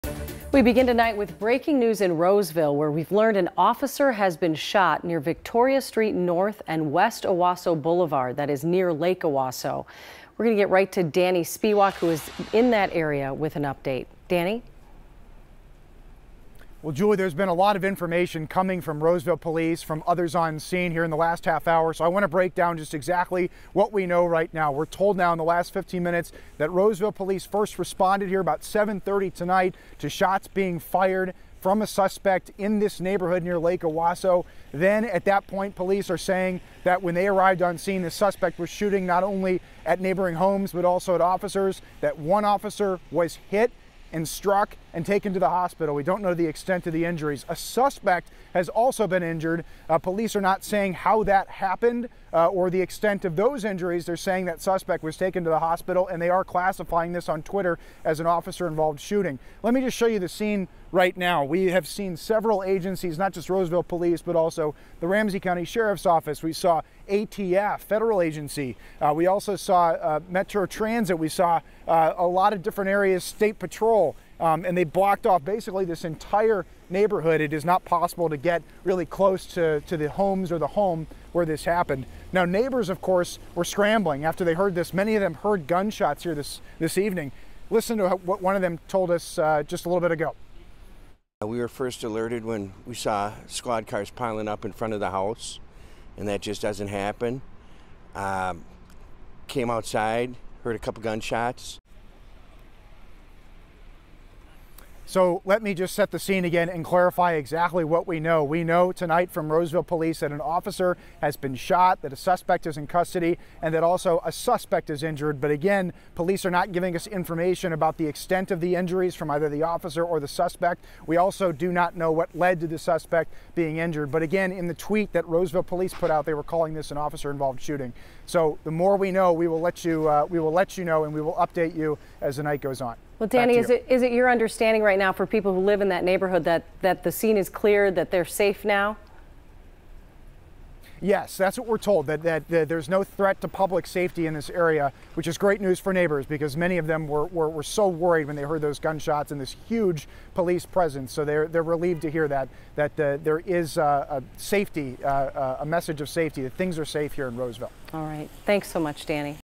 Officer shot, injured in Roseville
Roseville police say the officer was taken to the hospital, where the severity of their injuries is unknown. A suspect is currently in custody.